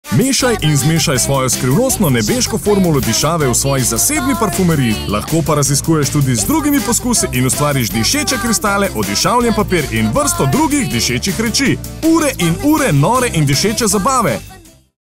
Wild! Science - Perfect Perfume Lab - TVC 15 sek SLO
Perfect Perfume Lab
Šifra izdelka: 410001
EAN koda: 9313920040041
Dimenzije: 25x25x7 cm
Opis:
Izdelaj si skrivni recept za proizvodnjo unikatne dišave! Uporabi laboratorijske pripomočke za izvleči, filtrirati in zmešati številne različne vonjave.
Spoznaj različne ekstrakte, emulzije, postopek filtriranja pod tlakom in zmešaj številne vonjave, ter si tako izdelaj lastno nepozabno dišavo, ki je lahko nepozabno darilo za prijateljice in sorodnike.
Vzgajaj barvite, dišeče gel kristale in izdelaj zabavne parfumirane glene in veliko drugega!

Primerno za starost: +8 let